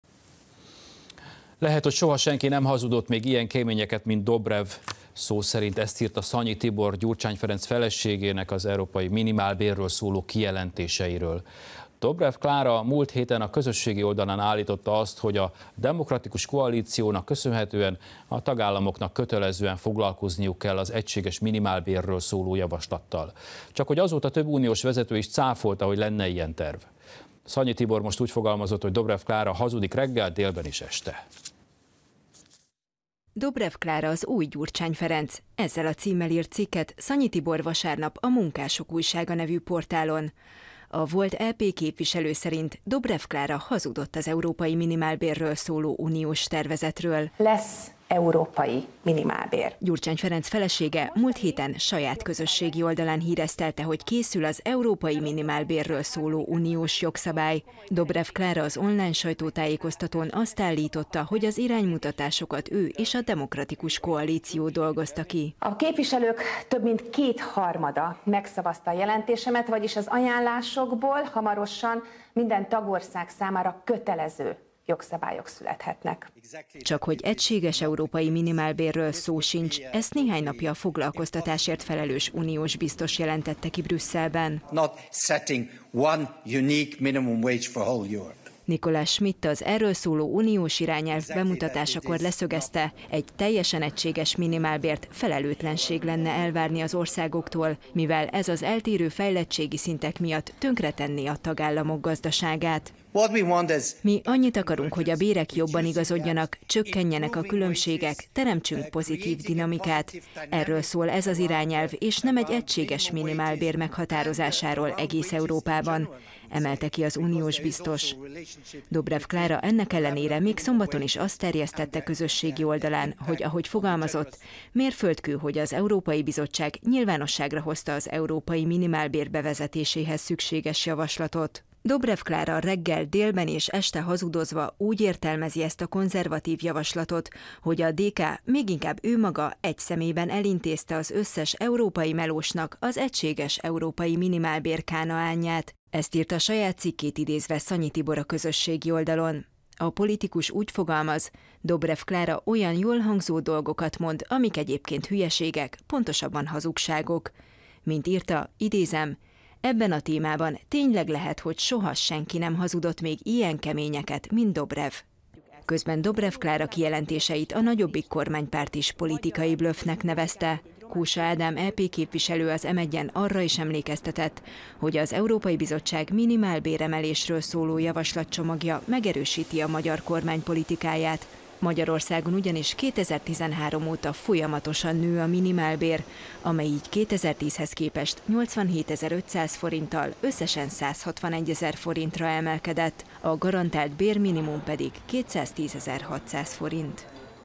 Szanyi Tibor: Dobrev Klára hazudott
Lehet, hogy soha senki nem hazudott még ilyen keményeket, mint Dobrev - szó szerint ezt írta Szanyi Tibor Gyurcsány Ferenc feleségének az európai minimiálbérről szóló kijelentéseiről. Dobrev Klára múlt héten a közösségi oldalán állította azt, hogy a Demokratikus Koalaíciónak köszönhetően a tagállamoknak kötelezően foglalkozniuk kell az egységes minimálbérről szóló javaslattal. Csakhogy azóta több uniós vezető is cáfolta, hogy lenne ilyen terv. Szanyi Tibor most úgy fogalmazott: Dobrev Klára hazudik reggel, délben és este.